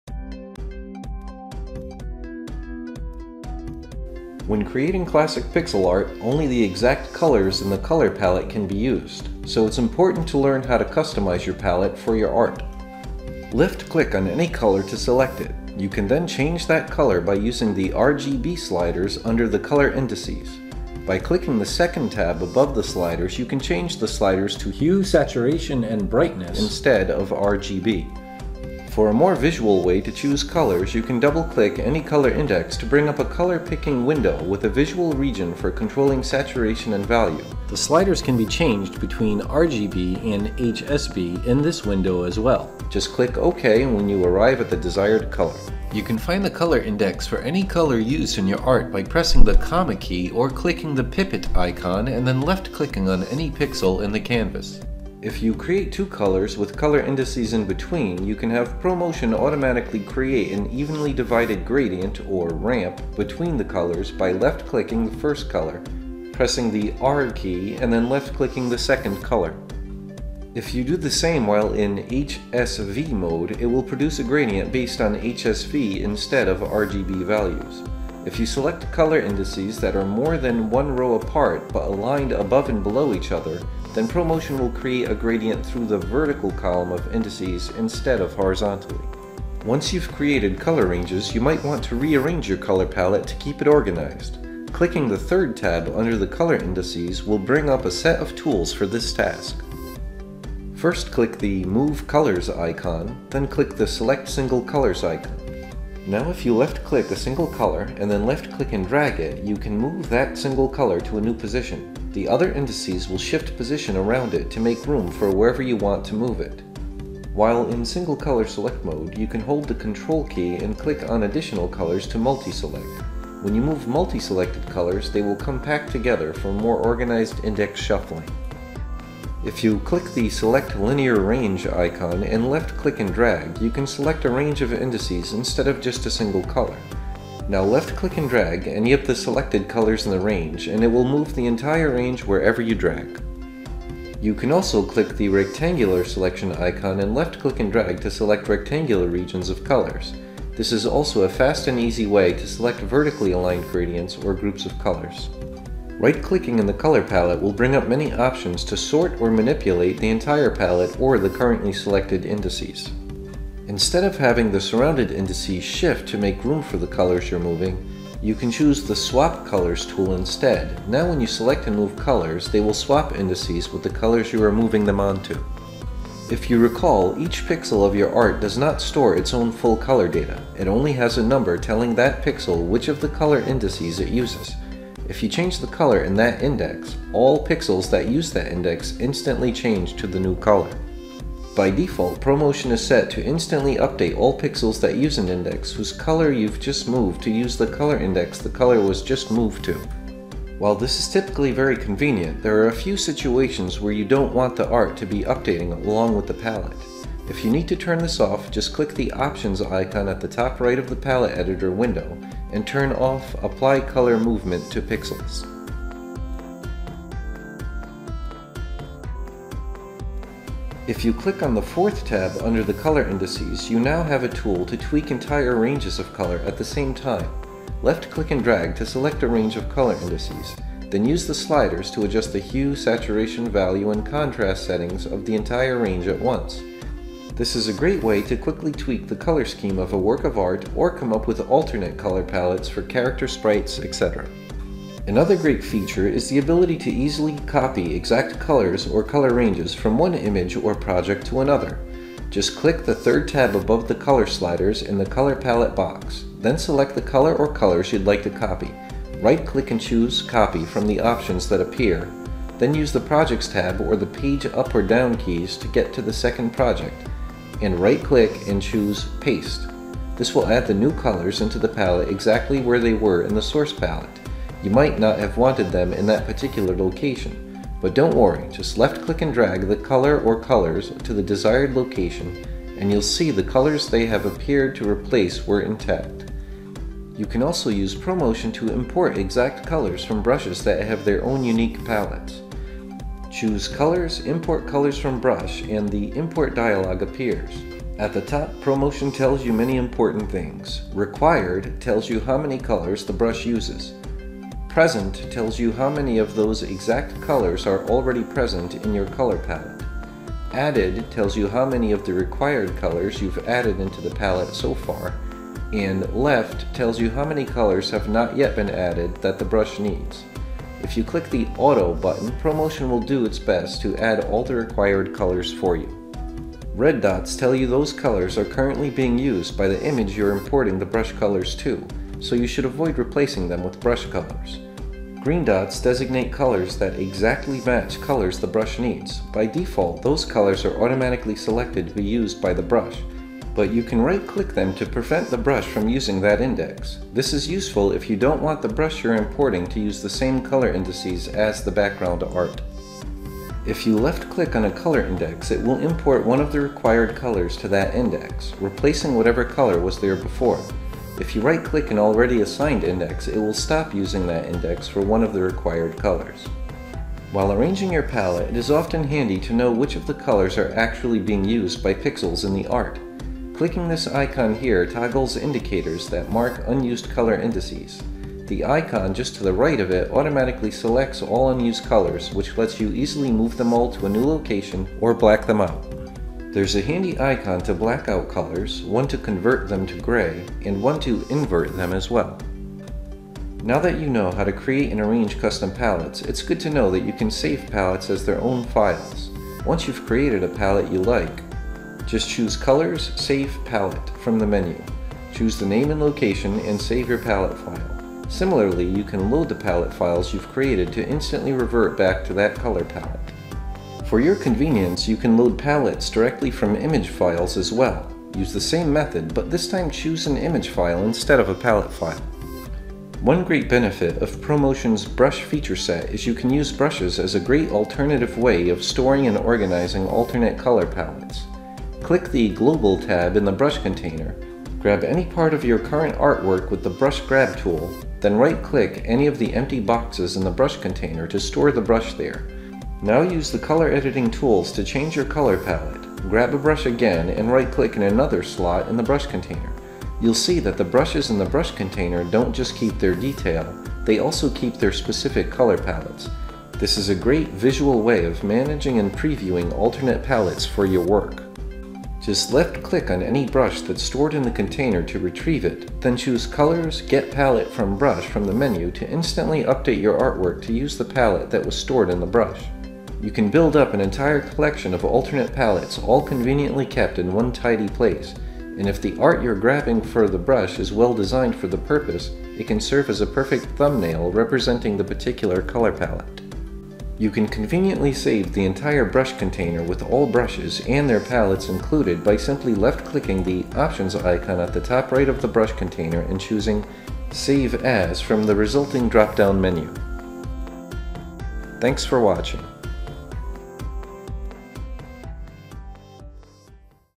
Using Color Palettes in Pro Motion NG
This tutorial shows you the basics of editing and arranging color palettes in Pro Motion NG
Learn how to:
- arrange colors in the palette
- create ramps
- copy, move colors
- change saturation or contrast
- transferring colors from project to project or brush to project